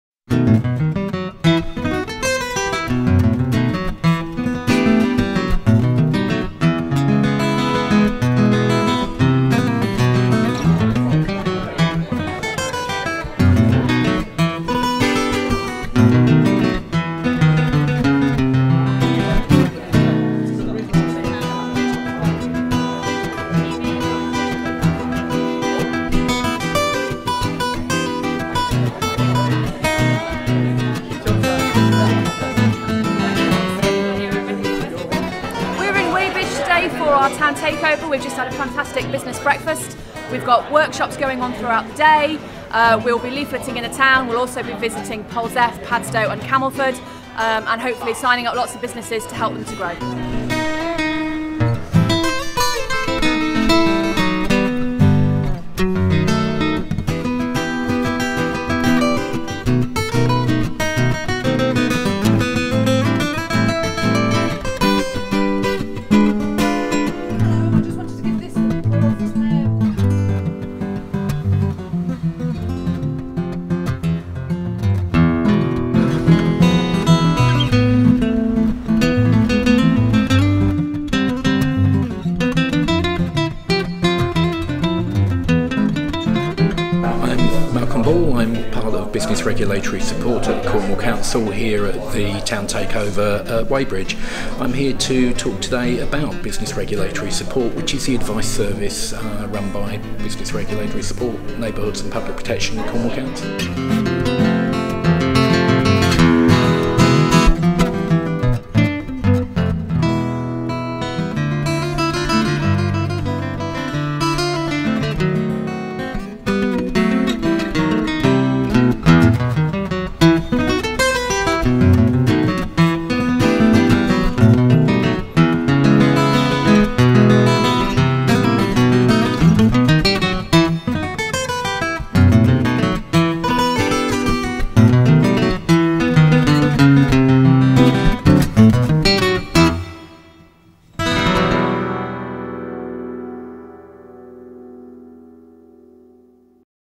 Wadebridge#TownTakeover
The Growth Hub were out and about in Wadebridge on Wednesday 18th October to speak to local businesses about business support.